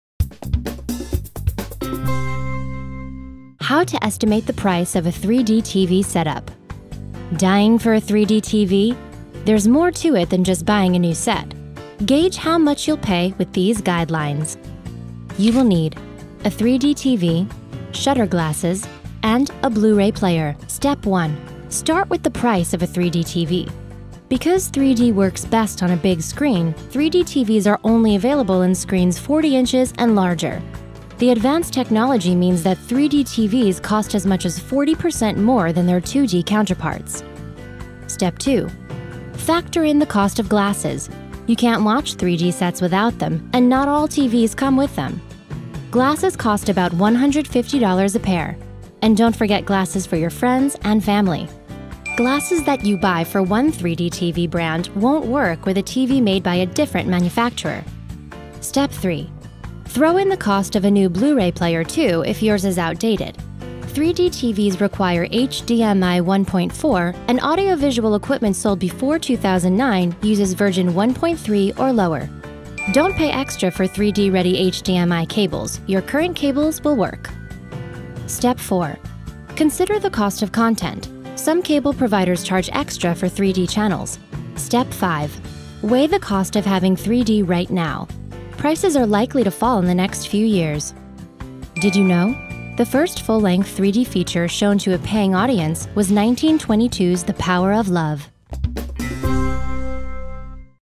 How to Estimate the Price of a 3-D TV Setup
Dying for a 3-D TV? There's more to it than just buying a new set. Gauge how much you'll pay with these guidelines.

Step 1: Start with the price of the TV
Start with the price of a 3-D TV: because 3-D works best on a big screen, 3-D TVs are only available in screens 40 inches and larger. The advanced technology means that 3-D TVs cost as much as 40 percent more than their 2-D counterparts.

Step 2: Factor in the glasses
Factor in the cost of glasses; you can't watch 3-D sets without them, and not all TVs come with them. Glasses cost about $150 a pair. And don't forget glasses for your friends and family.

Tip
Glasses that you buy for one 3-D TV brand won't work with a TV made by a different manufacturer.

Step 3: Throw in a Blu-ray player
Throw in the cost of a new Blu-ray player, too, if yours is outdated. 3-D TVs require HDMI 1.4, and audiovisual equipment sold before 2009 uses version 1.3 or lower.

Tip
Don't pay extra for 3-D-ready HDMI cables; your current cables will work.

Step 4: Consider the cost of content
Consider the cost of content: some cable providers charge extra for 3-D channels.

Step 5: Weigh the cost of waiting
Weigh the cost of having 3-D right now. Prices are likely to fall in the next few years.

The first full-length 3-D feature shown to a paying audience was 1922's The Power of Love.